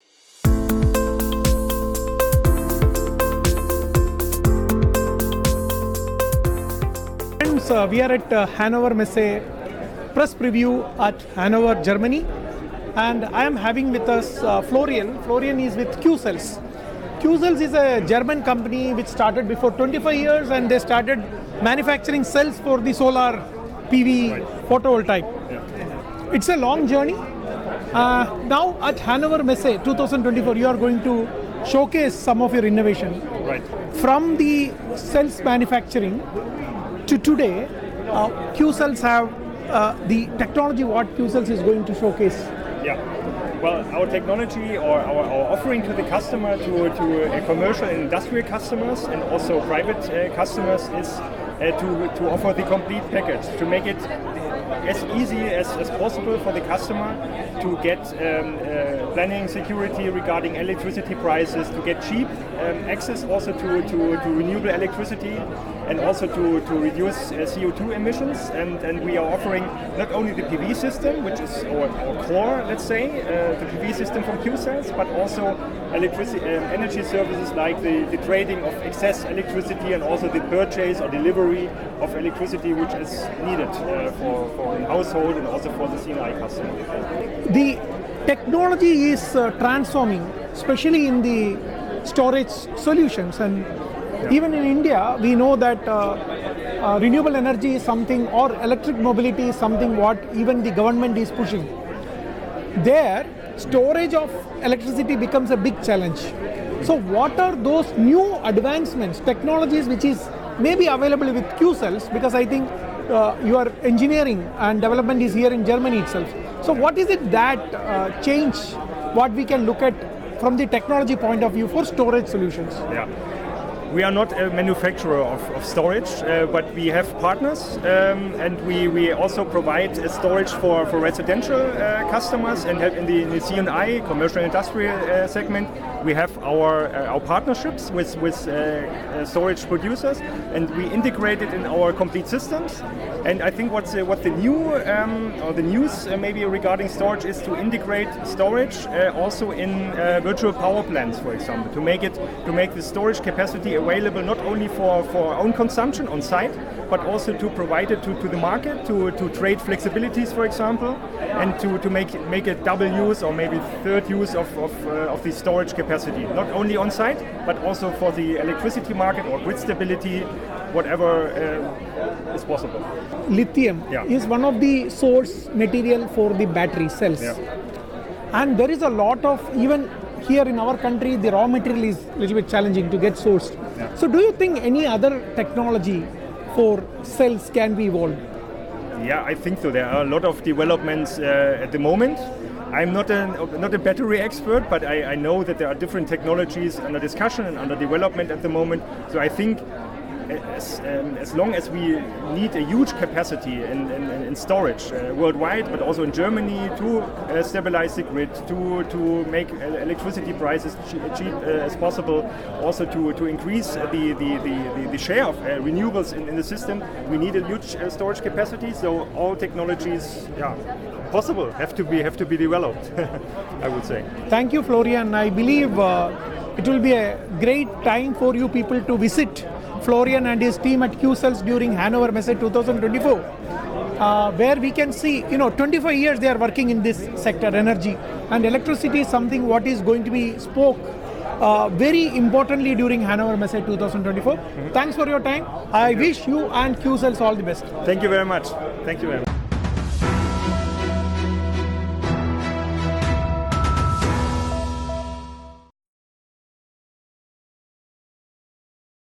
Empowering Sustainability | Qcells' Solar Revolution at Hannover Messe 2024| Florian Schmidt
At Hannover Messe 2024, Qcells Europe is set to inspire with a powerful motto: "Think entrepreneurially. Act sustainably. Invest in photovoltaics now." This rallying cry is crucial as Europe grapples with an unprecedented energy crisis. Qcells is at the forefront, advocating for solar power as a resilient solution that secures the future of businesses and promises independence and significant savings on energy costs.

During the Hannover Messe 2024 Press Preview on February 21st, Hari Shanker, the Managing Editor of Machine Maker, caught up with Florian Schmidt, the Business Development Manager for Energy Solutions at Qcells Europe. Their conversation shed light on Qcells' 25-year innovation journey in renewable energy, topics like battery storage, and exploring alternatives beyond lithium.

Photovoltaic (PV) systems offer one of the most cost-effective methods for local electricity generation. Qcells emphasises the dual benefit of investing in PV systems: they guarantee stable, predictable electricity prices for decades and propel the shift towards a more climate-friendly economy.

Qcells talks about providing comprehensive energy solutions tailored to each customer's unique needs and conditions. Their portfolio includes custom-designed PV systems, electricity storage units, charging stations, and electricity tariffs, all aimed at empowering customers to craft their own sustainable power supply.

As Hannover Messe 2024 approaches, Qcells is ready to showcase its commitment to supporting businesses every step towards sustainable energy production. From professional planning and high-quality components to direct marketing, attractive operational models, and straightforward payment options, they are prepared to lead the charge in the solar revolution.

Watch Machine Maker for more updates from the exciting manufacturing world and the groundbreaking revelations at Hannover Messe 2024.